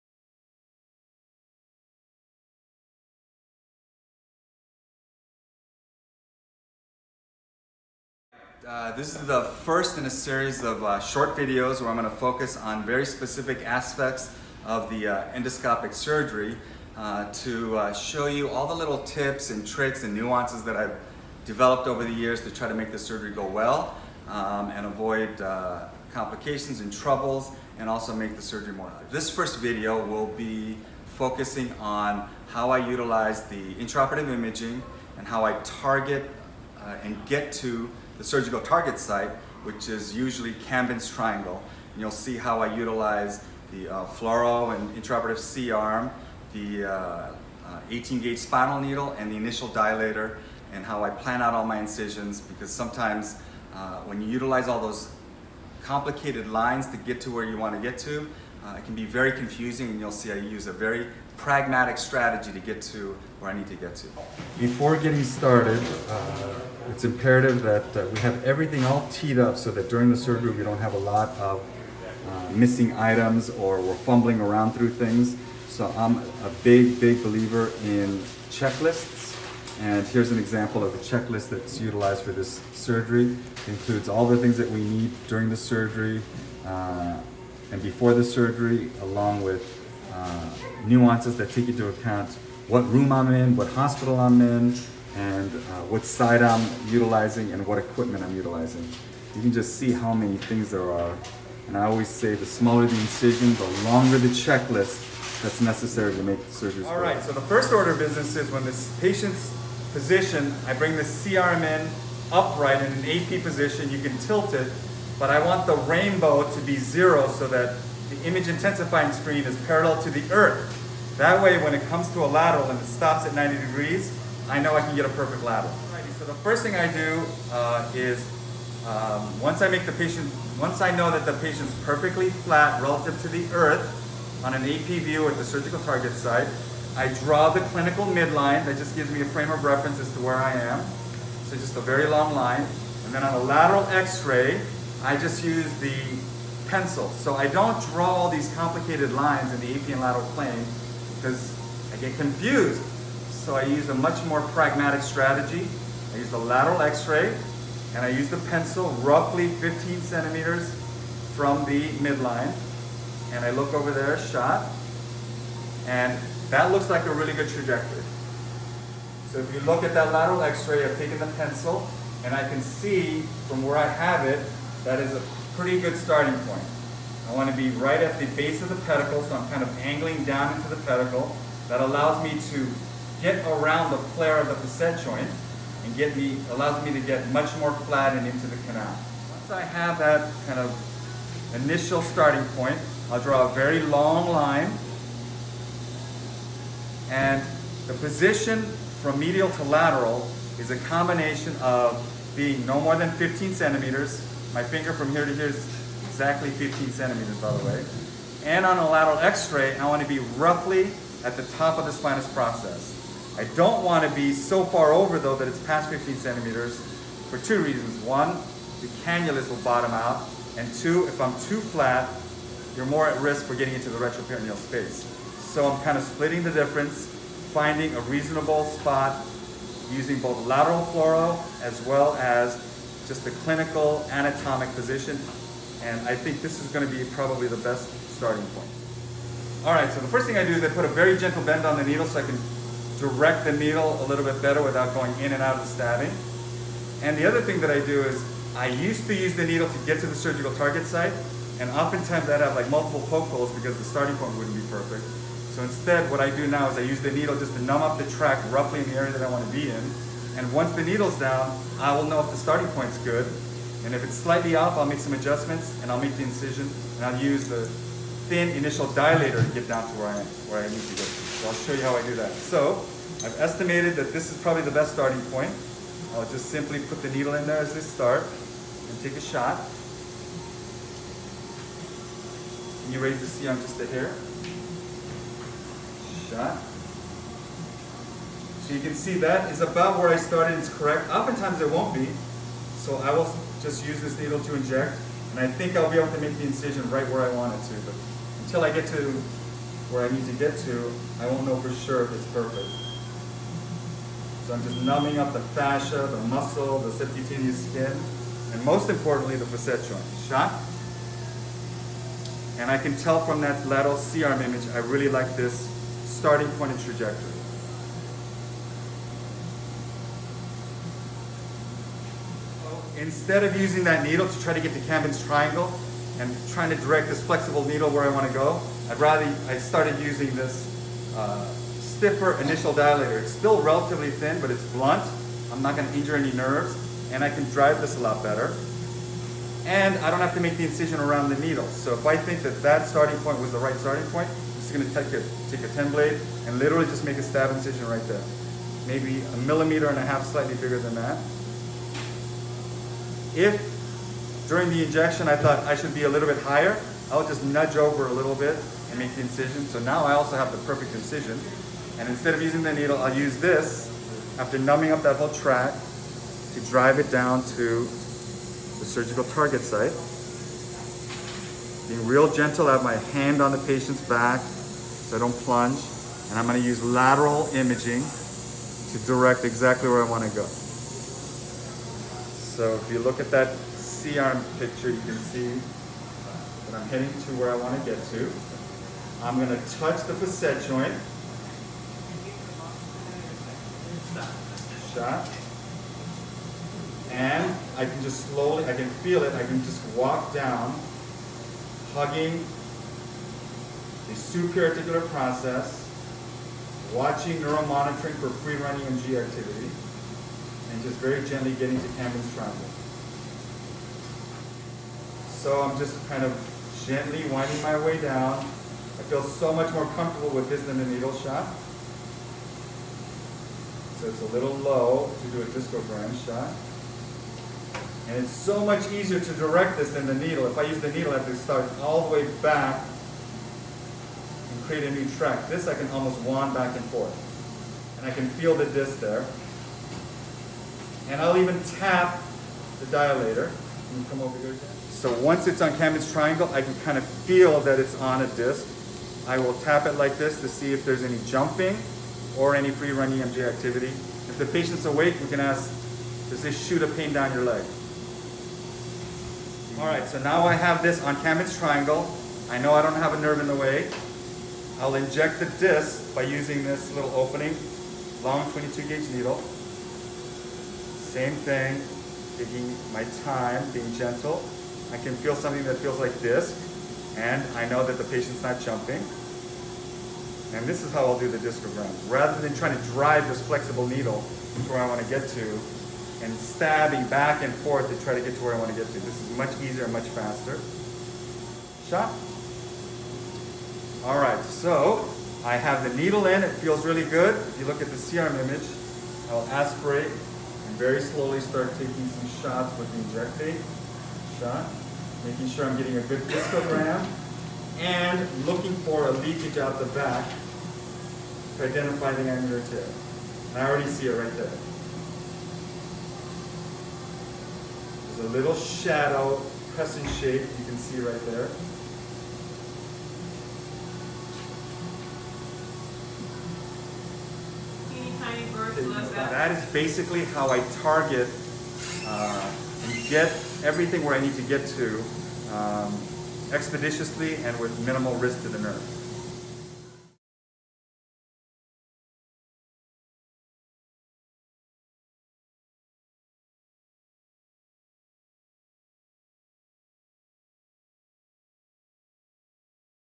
How I use intraoperative imaging to get to the working triangle.
Advanced Techniques in LESS (Laser Endoscopic Spine Surgery): My tips and tricks on how I get to Kambin’s triangle with making multiple pokes and keeping the nerve safe.